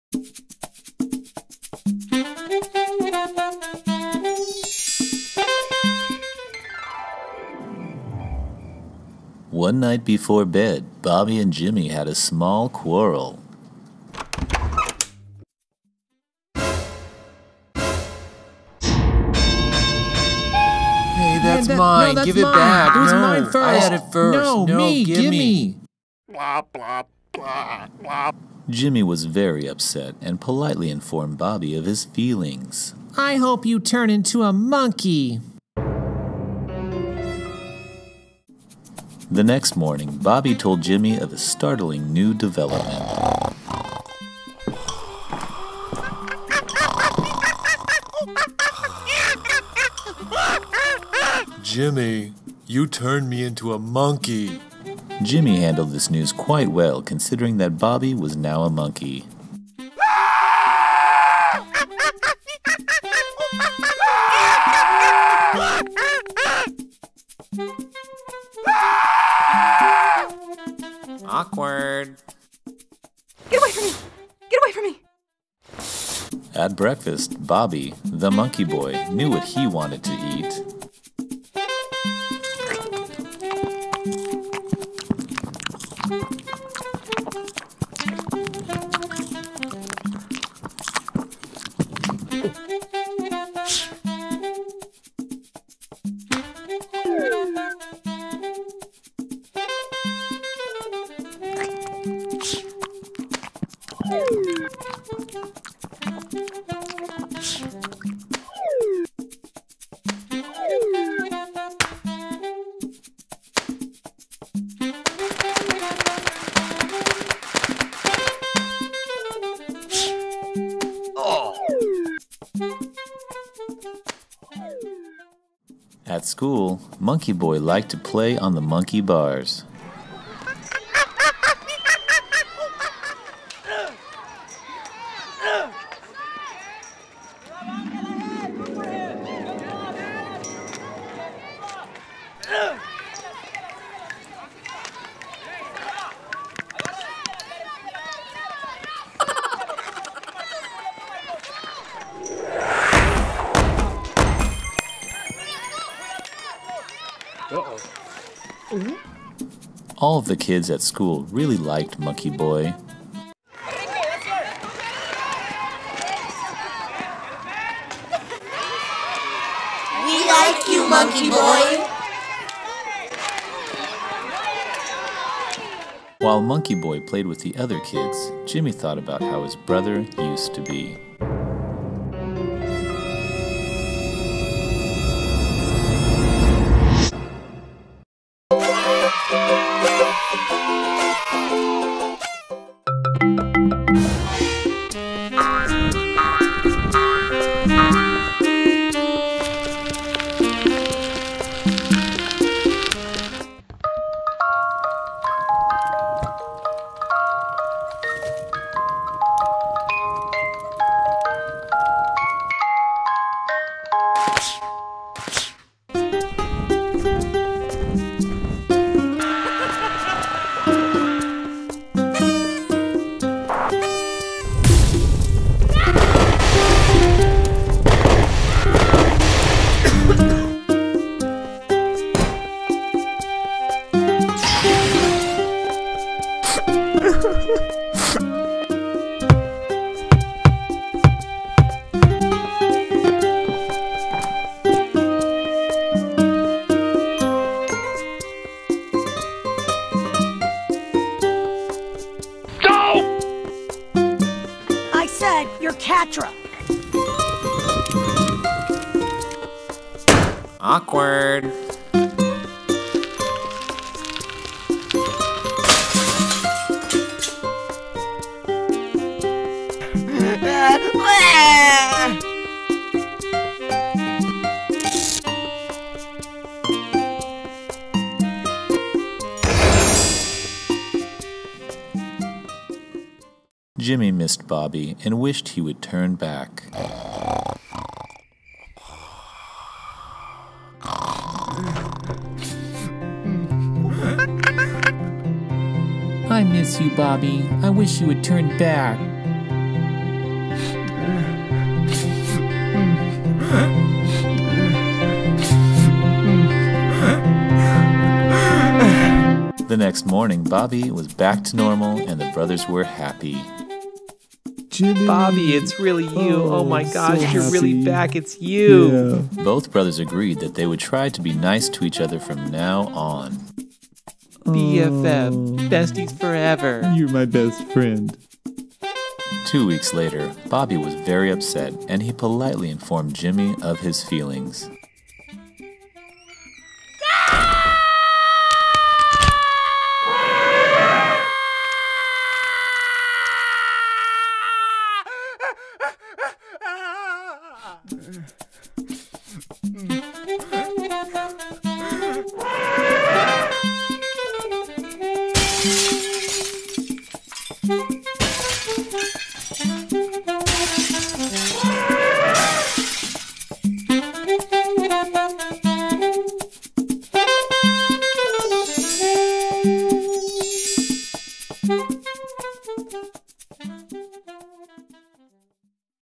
The Monkeyboy - STEAM Academy @ La Presa 2016
Final project for the Animation and Music Technology classes - STEAM Academy @ La Presa